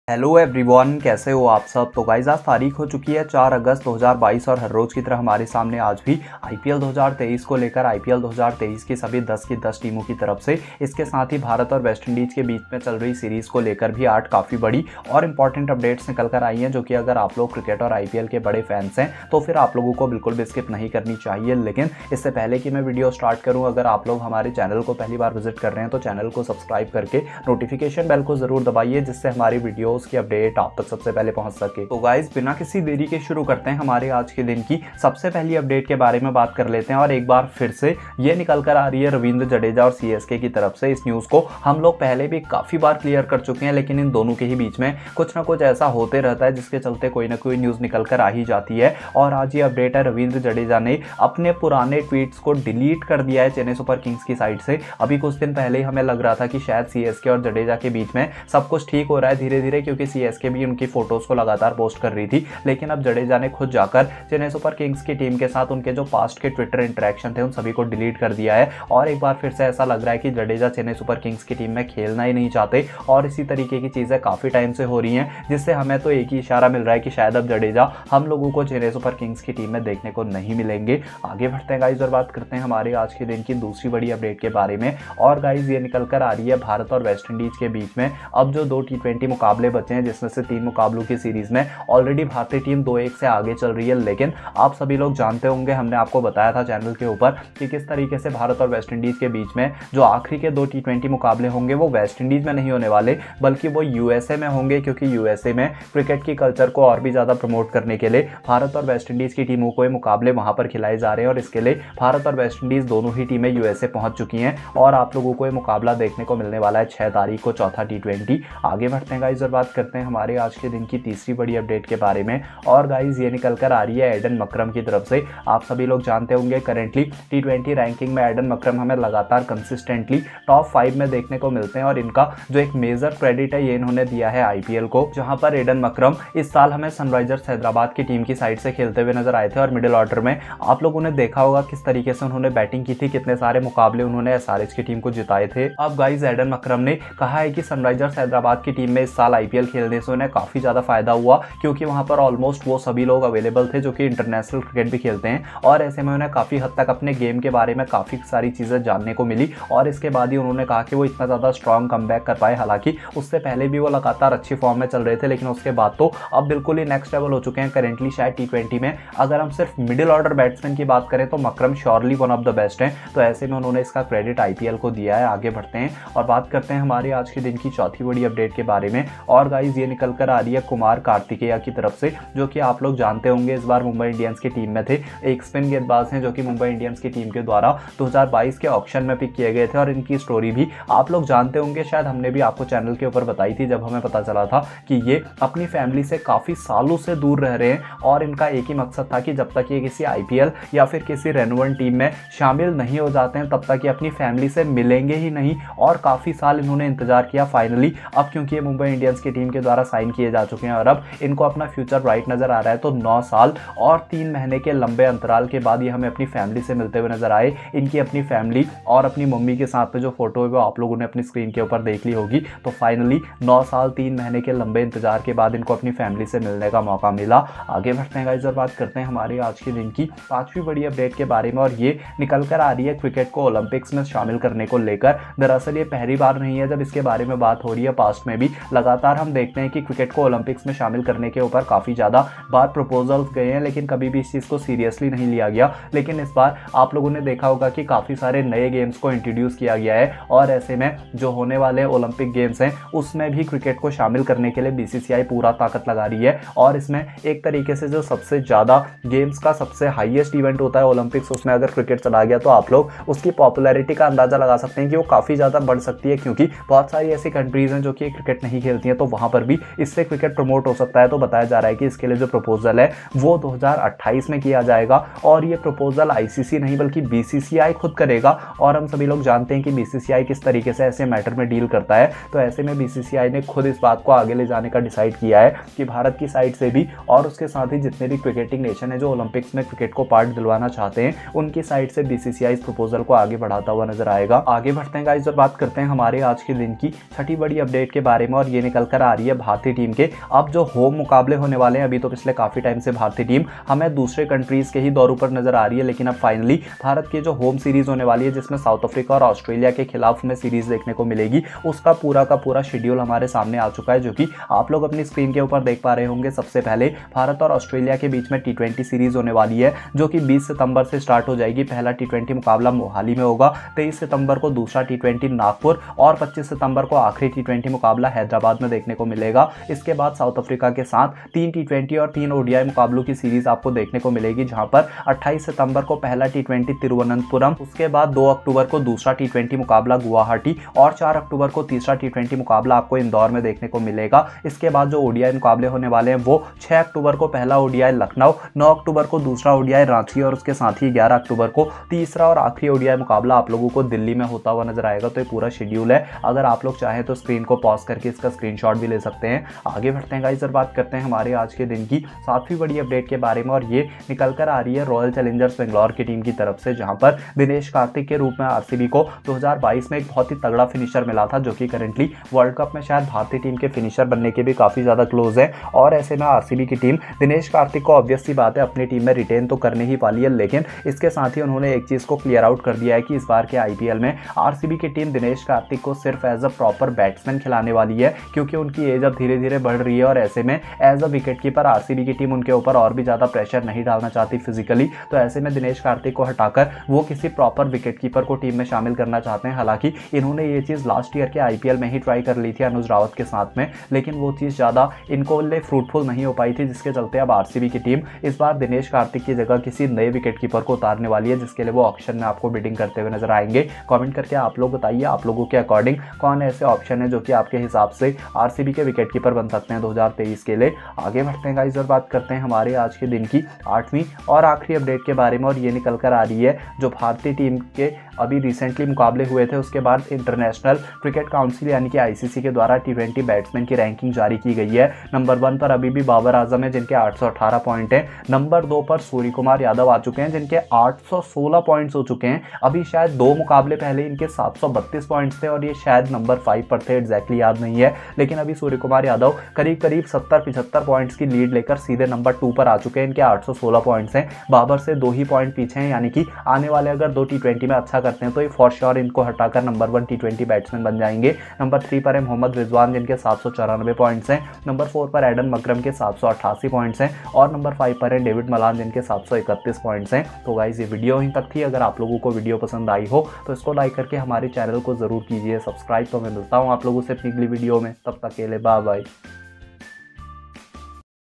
IPL 2023 - 8 BIG News For IPL on 4 Aug (5 BIG Releases, 9 Years Without? Jadeja Deletes, Ind vs WI)
Must Watch